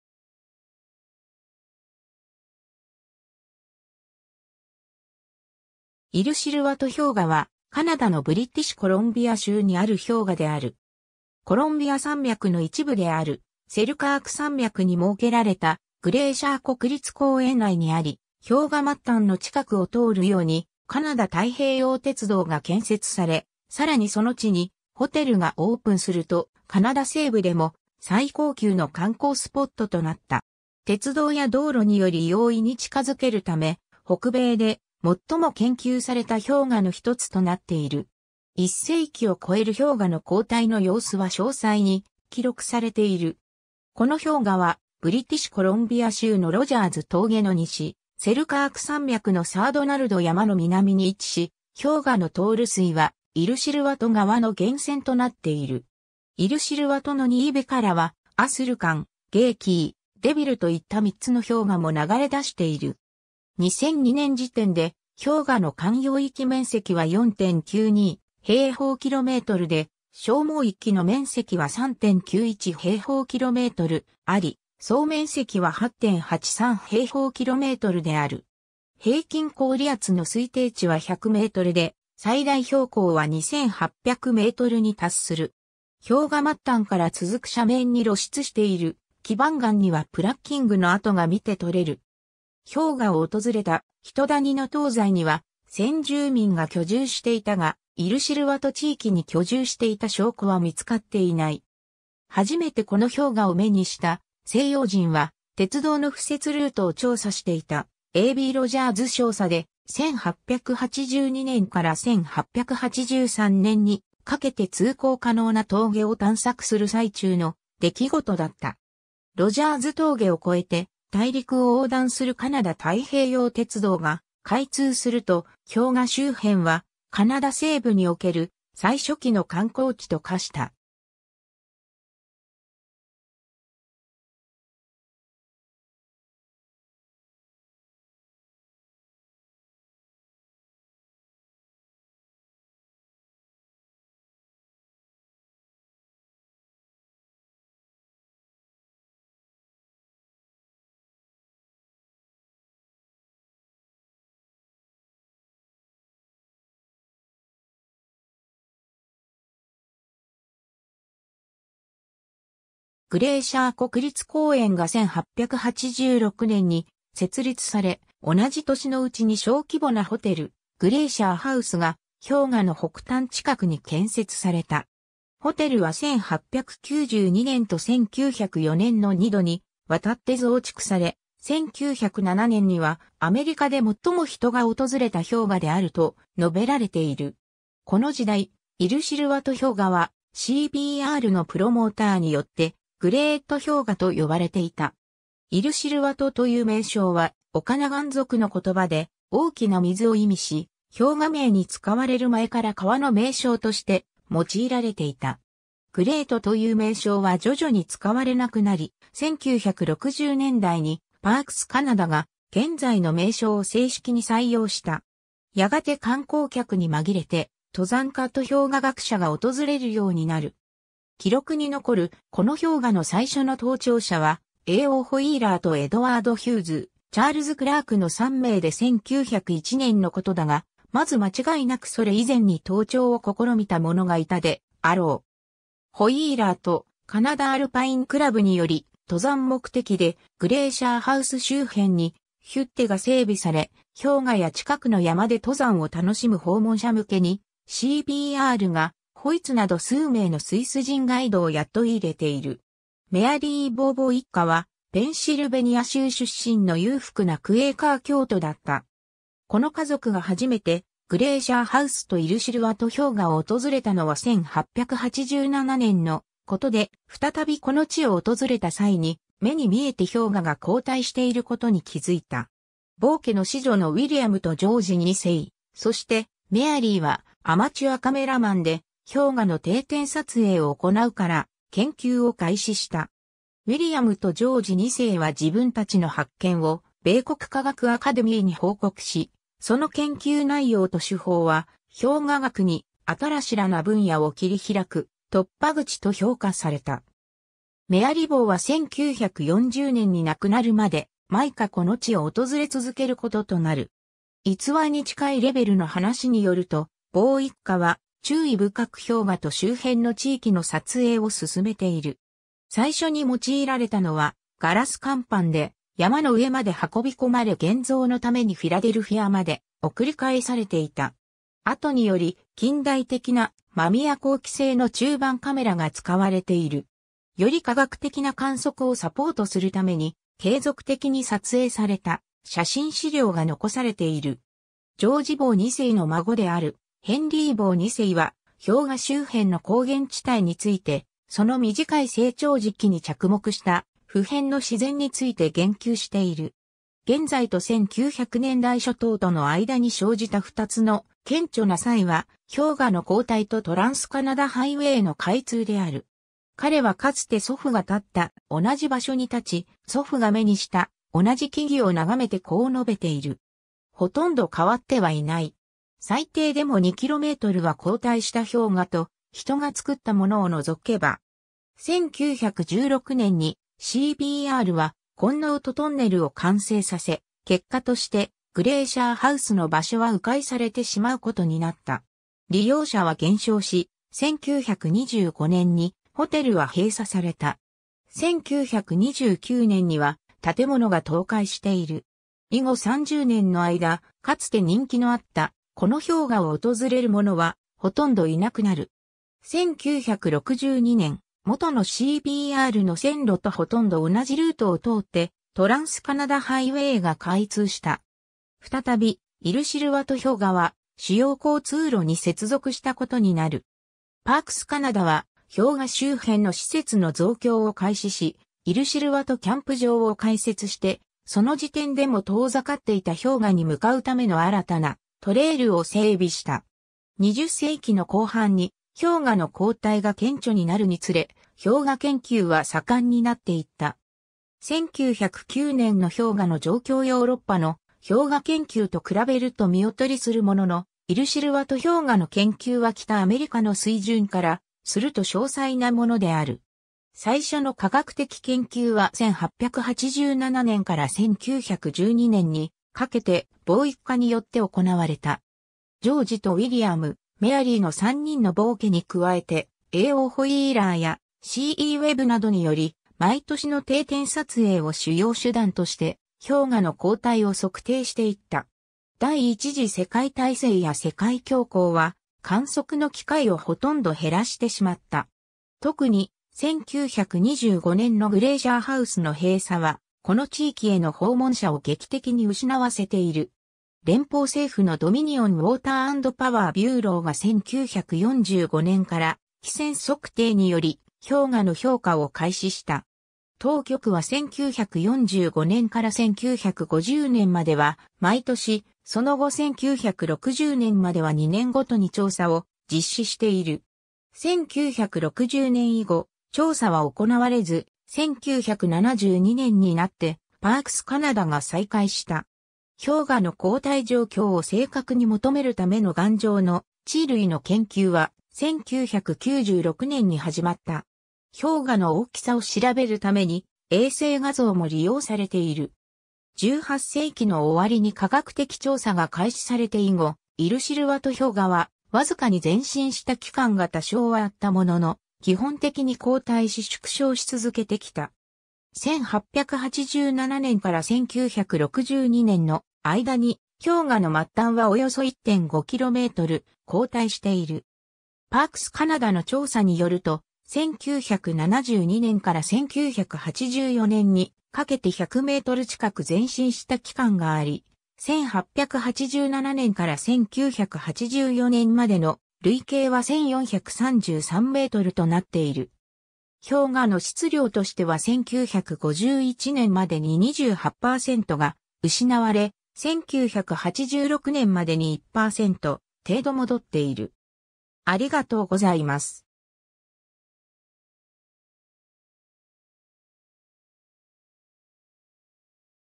イルシルワト氷河
イルシルワト氷河 （英: Illecillewaet Glacier、[ˌɪləˈsɪləwət] ）はカナダのブリティッシュ・コロンビア州にある氷河である。
コロンビア山脈の一部であるセルカーク山脈に設けられたグレーシャー国立公園内にあり、氷河末端の近くを通るようにカナダ太平洋鉄道 (CPR) が建設され、さらにその地にホテルがオープンすると、カナダ西部でも最高級の観光スポットとなった。
鉄道や道路により容易に近づけるため、北米で最も研究された氷河の一つとなっている。
1世紀を超える氷河の後退の様子は詳細に記録されている。
 この氷河はブリティッシュ・コロンビア州のロジャーズ峠の西、セルカーク山脈のサー・ドナルド山の南に位置し、氷河の融水はイルシルワト川の源泉となっている。
イルシルワトのニーヴェからは、アスルカン、ゲイキー、デビルといった３つの氷河も流れだしている 。
 2002年時点で、氷河の涵養域面積は 4.92 平方キロメートルで、消耗域 の面積は 3.91 平方キロメートルあり、総面積は 8.83 平方キロメートルである 。
 平均氷厚の推定値は 100 メートルで、最大標高は 2,800 メートルに達する。
氷河末端から続く斜面に露出している基盤岩にはプラッキングの跡が見て取れる。
 氷河を訪れた人 （1902年） 谷の東西には先住民が居住していたが、イルシルワト地域に居住していた証拠は見つかっていない 。
 初めてこの氷河を目にした西洋人は、鉄道の敷設ルートを調査していた A. B. ロジャーズ少佐で、 1882年から1883年にかけて通行可能な峠を探索する最中の出来事だった 。
 ロジャーズ峠を越えて大陸を横断するカナダ太平洋鉄道が開通すると、氷河周辺はカナダ西部における最初期の観光地と化した。
 グレーシャー国立公園が1886年に設立され、同じ年のうちに小規模なホテル、グレーシャー・ハウスが氷河の北端近くに建設された。
 ホテルは1892年と1904年の2度にわたって増築され 、1907年には「アメリカで最も人が訪れた氷河である」と述べられている。
 この時代、イルシルワト氷河は、CPRのプロモーターによって「グレート氷河」と呼ばれていた。
“イルシルワト (Illecillewaet) ” という名称は、オカナガン族の言葉で「大きな水」を意味し、氷河名に使われる前から川の名称として用いられていた 。
 「グレート」という名称は徐々に使われなくなり、1960年代にパークス・カナダが現在の名称を正式に採用した。
 やがて観光客に紛れて、登山家と氷河学者が訪れるようになる。
記録に残るこの氷河の最初の登頂者は、A.O. ホイーラー とエドワード・ヒューズ、チャールズ・クラークの3名で1901年の事だが、まず間違いなくそれ以前に登頂を試みた者がいたであろう 。
 ホイーラーとカナダ・アルパイン・クラブにより、登山目的でグレーシャー・ハウス周辺にヒュッテが整備され、氷河や近くの山で登山を楽しむ訪問者向けに、CPR がフォイツなど数名のスイス人ガイドを雇い入れている。
 メアリー・ヴォー （1914年撮影） ヴォー一家はペンシルベニア州出身の裕福なクエーカー教徒だった。
この家族が初めてグレーシャー・ハウスとイルシルワト氷河を訪れたのは1887年のことで、再びこの地を訪れた際に、目に見えて氷河が後退していることに気づいた。
ヴォー家の子女のウィリアムとジョージ2世、そしてメアリー（後のメアリー・ヴォー・ウォルコット）はアマチュアカメラマンで、氷河の定点撮影を行うから研究を開始した。
ウィリアムとジョージ2世は自分たちの発見を米国科学アカデミーに報告し、その研究内容と手法は、氷河学に新らな分野を切り開く「突破口」と評価された。
メアリ・ヴォーは1940年に亡くなるまで、毎夏この地を訪れ続けることとなる 。
 逸話に近いレベルの話によると、ヴォー一家は注意深く氷河と周辺の地域の撮影を進めている。
最初に用いられたのはガラス乾板で、山の上まで運び込まれ現像のためにフィラデルフィアまで送り返されていた。
後により近代的なマミヤ光機製の中判カメラが使われている。
より科学的な観測をサポートするために、継続的に撮影された写真資料が残されている。
ジョージ・ヴォー2世の孫である（山岳写真家でもあった）ヘンリー・ヴォー2世は、氷河周辺の高原地帯について、その短い成長時期に着目した普遍の自然について言及している。
現在と...